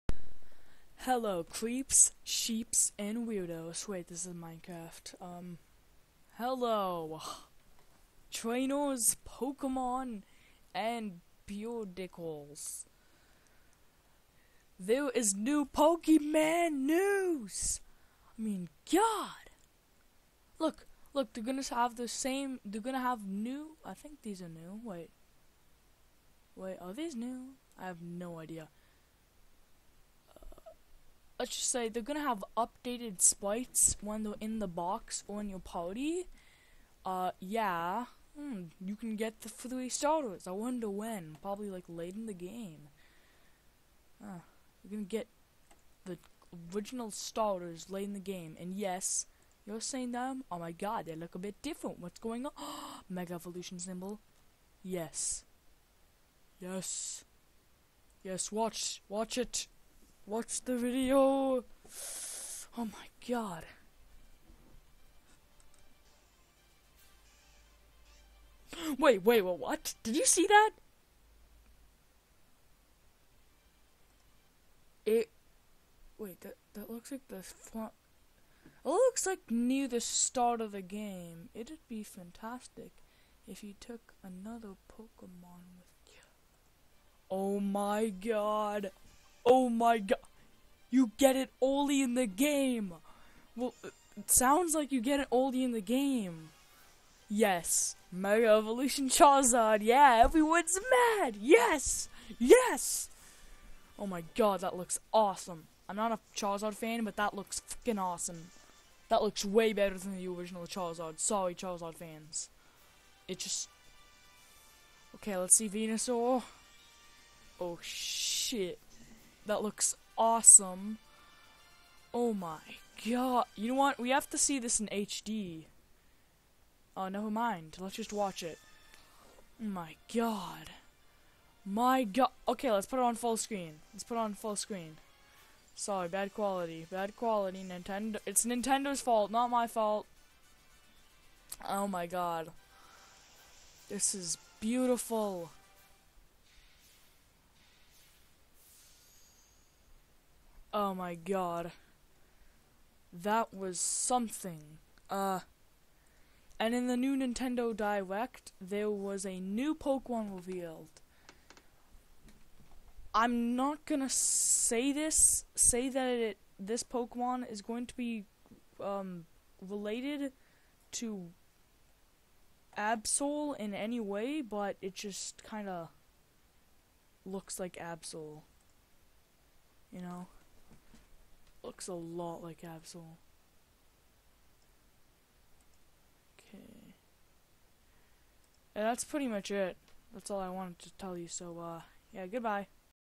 Pokemon: MOAR X AND Y NEWS?!?! Kanto Mega Evolutions! Starter Kanto Pokemon early in game?!
OMG new awesome leaked news and some new pokemon revealed ;'D crying so hard. with joy... LOVE IT! thanks Serebii.net for this info!